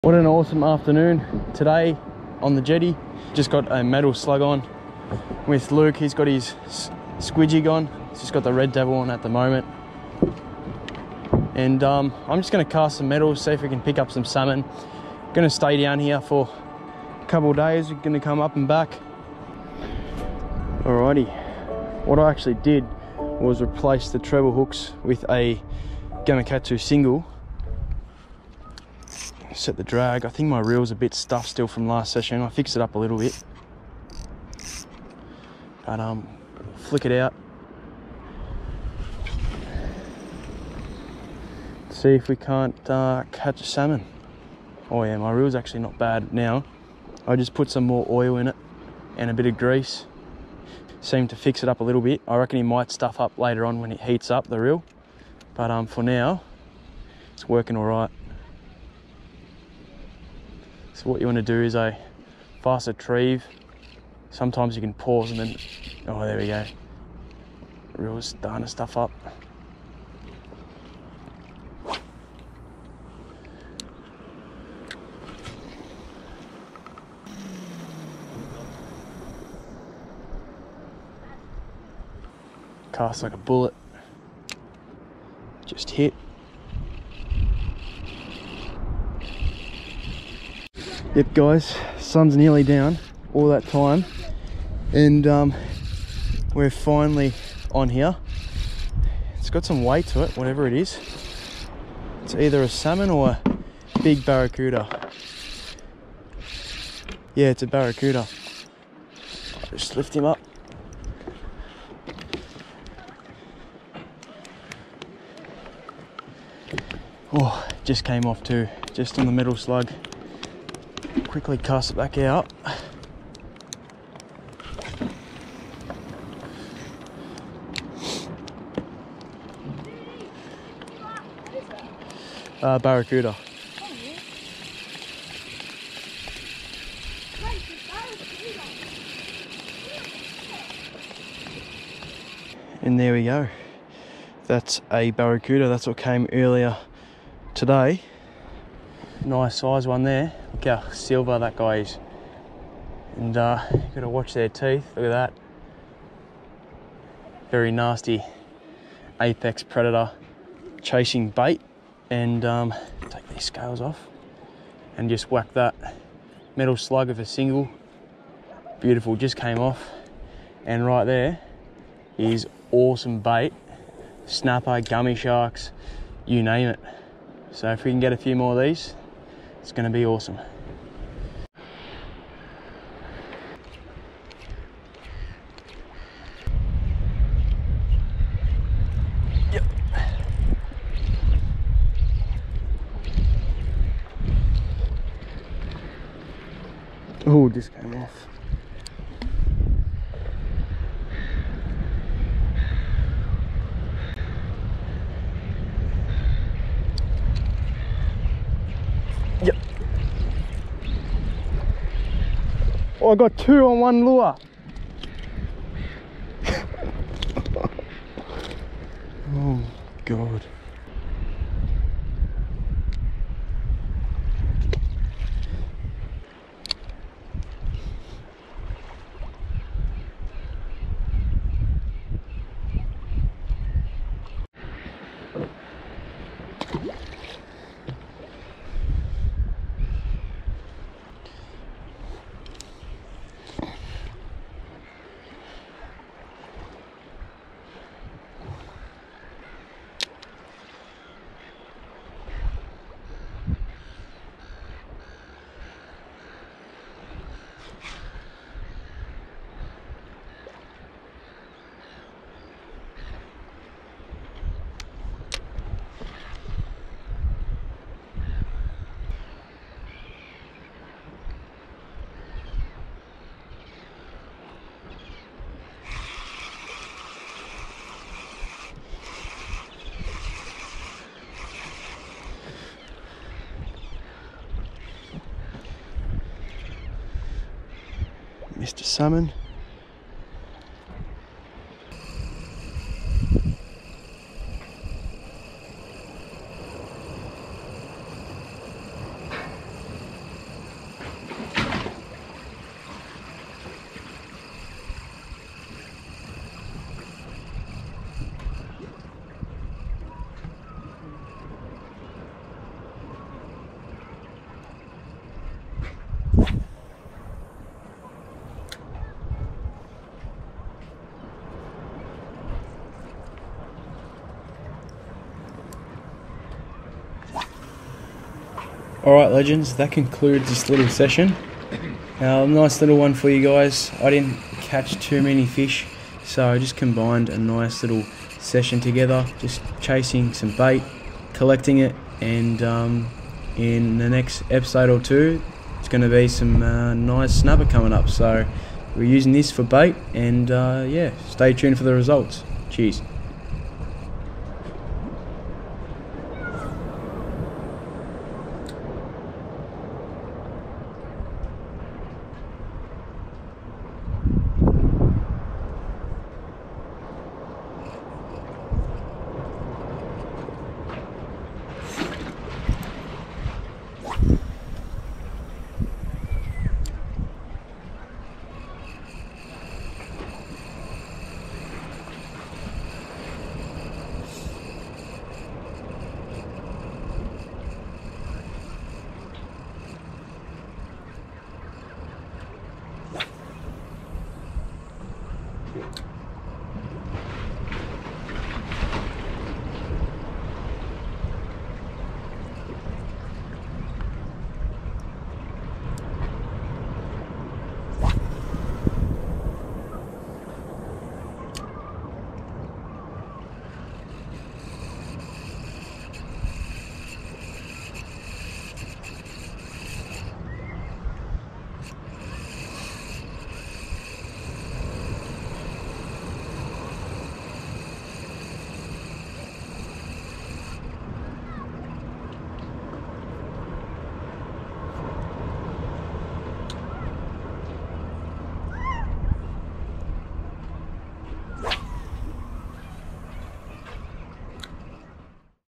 METAL LURE FISHING!
Metal lure fishing off the jetty can provide with some great fish!
WANT EARLY ACCESS TO EPISODES AND MORE!